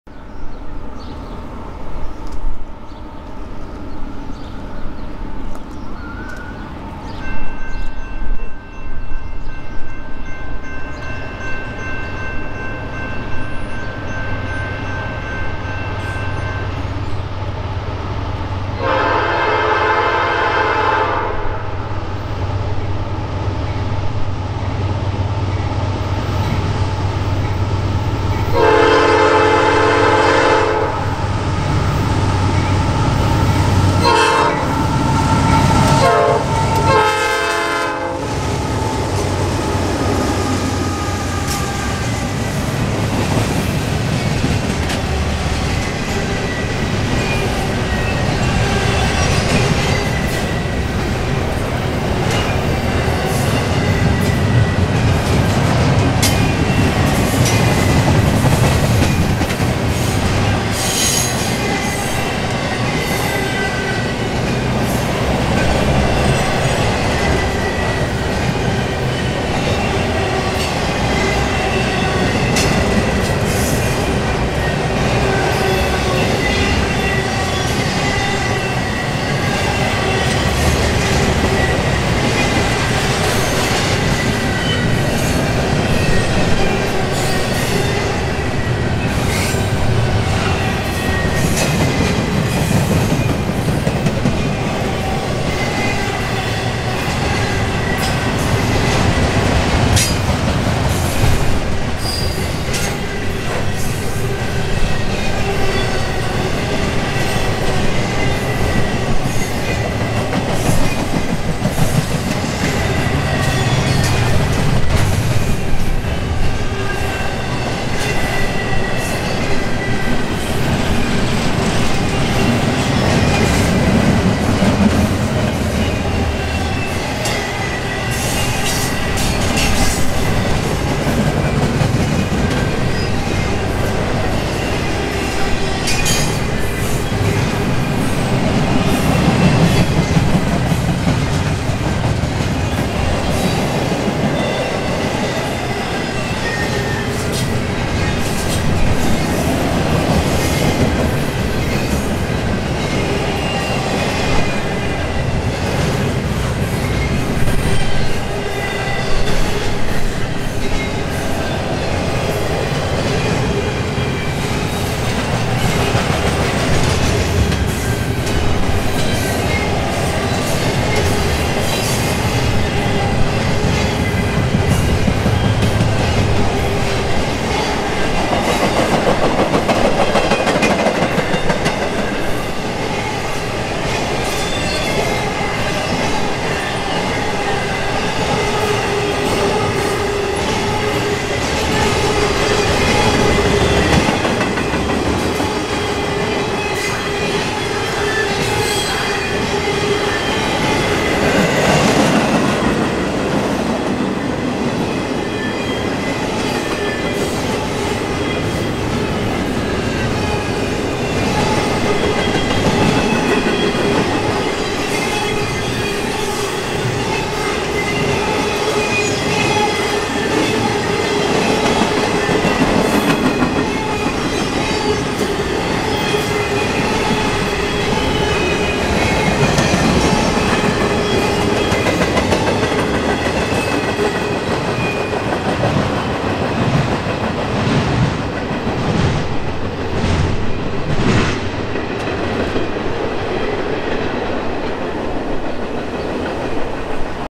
NORFOLK SOUTHERN GE AC44C6M Southbound Intermodal Container Train
A southbound Intermodal Container Lead by Norfolk Southern GE AC44C6M #4058 Engine with a Nathan P5 Horn passing through at Milepost Rosemont.